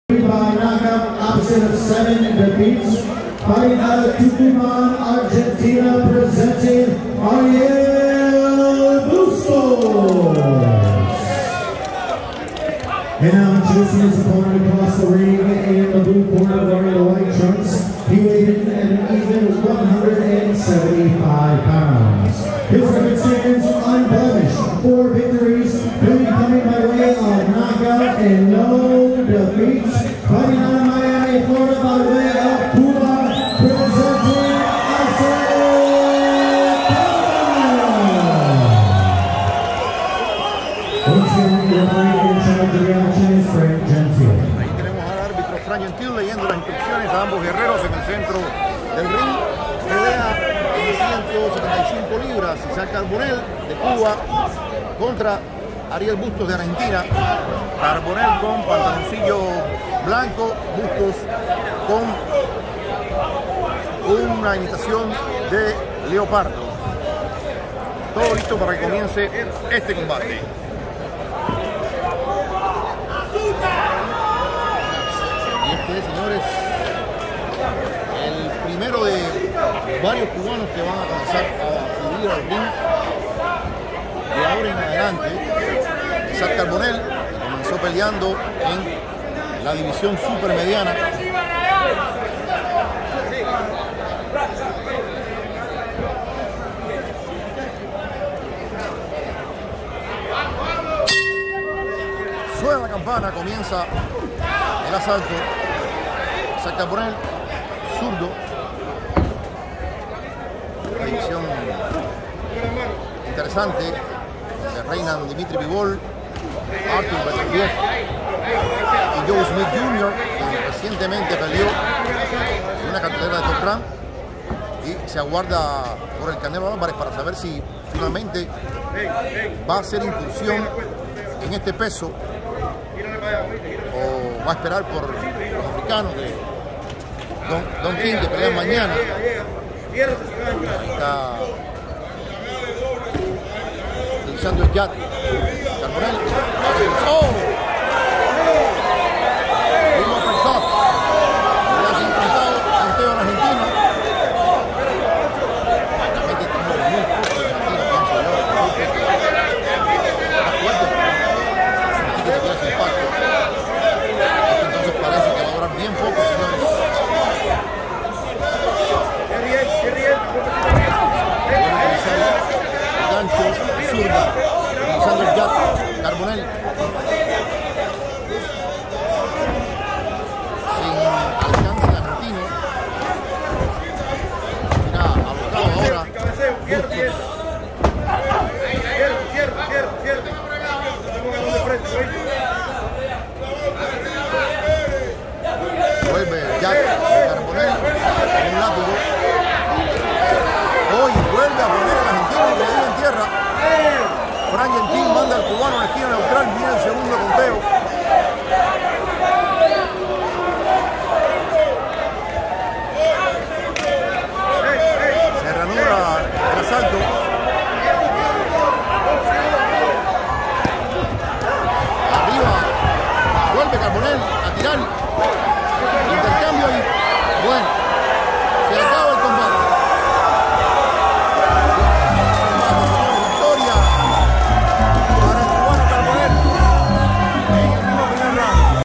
ISAAC CARBONELL: tres golpes y nos fuimos
Esta pelea tuvo lugar el viernes 28 de Enero en el Hialeah Park, en Miami, como parte de una velada de Warriors Boxing.